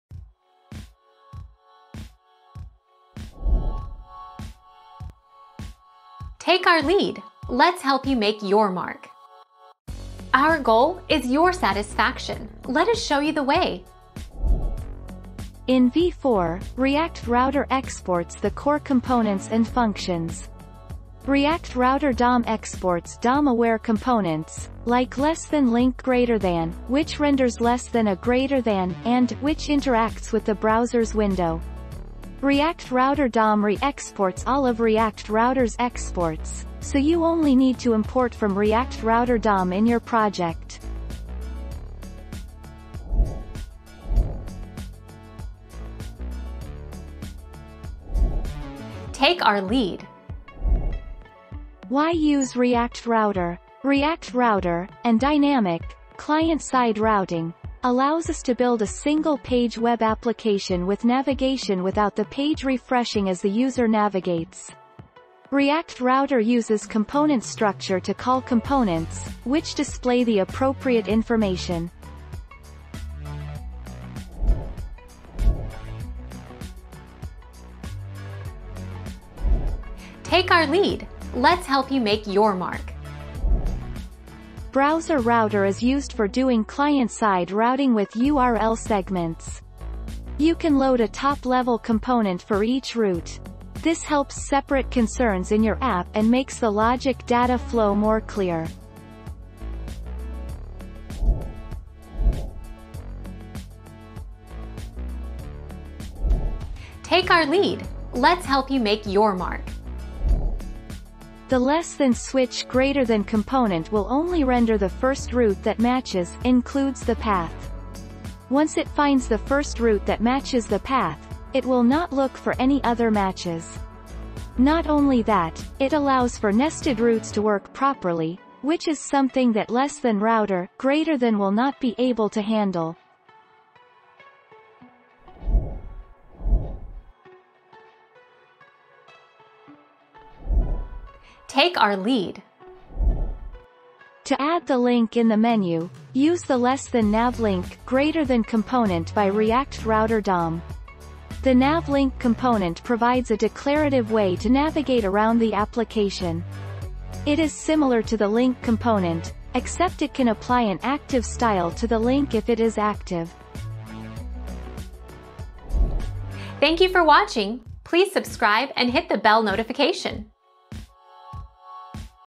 What is the difference between React router and react router Dom?
00:00 - What is the difference between React router and react router Dom?
00:45 - Why do we need react router?
01:16 - Why we use BrowserRouter in react?
01:42 - What is switch in router react?
02:16 - How do you link components in react?

Laura S. Harris (2021, January 10.) What is the difference between React router and react router Dom?
    AskAbout.video/articles/What-is-the-difference-between-React-router-and-react-router-Dom-204603